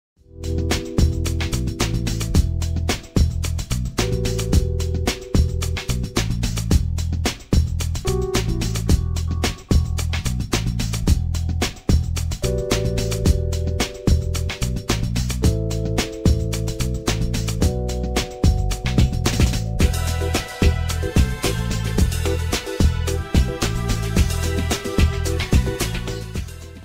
Trailer des JPO 2017
Vendredi 24 et samedi 25 février 2017, rendez-vous à la Manufacture des Oeillets, 1 place Pierre Gosnat 94200 Ivry-Sur-Seine
Journées Portes Ouvertes de l'Ecole professionnelle supérieure d'arts graphiques de la Ville de Paris, école d'art publique accessible sur concours (atelier prépa, cursus en design graphique et digital media)